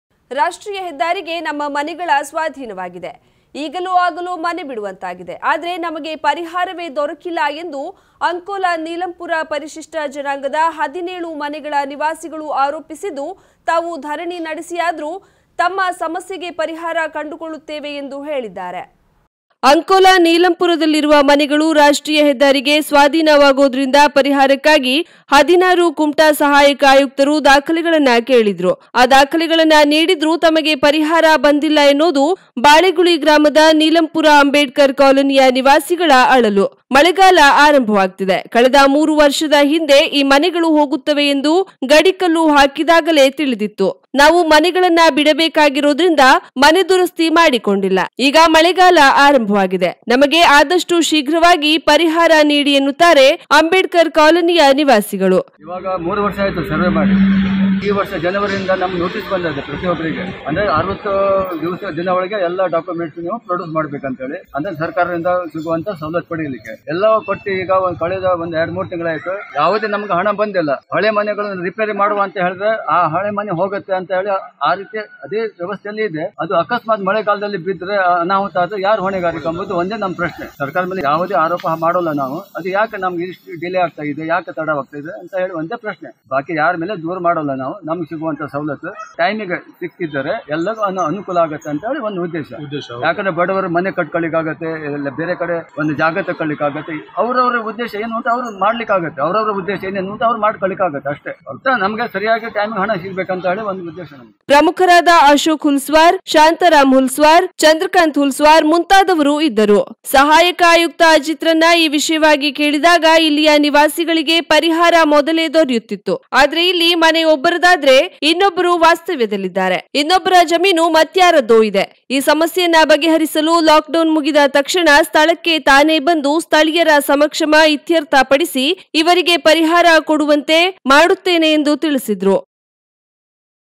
ನೀಲಂಪುರ ನಿವಾಸಿಗಳಿಗೆ ಸಿಗದ ಪರಿಹಾರ | ಚತುಷ್ಪತ ಹೆದ್ದಾರಿಗೆ ತೆರವಾಗುವ ಮನೆಗಳು
Solution for the residents of Neelampur | Homes to the quadrangular highway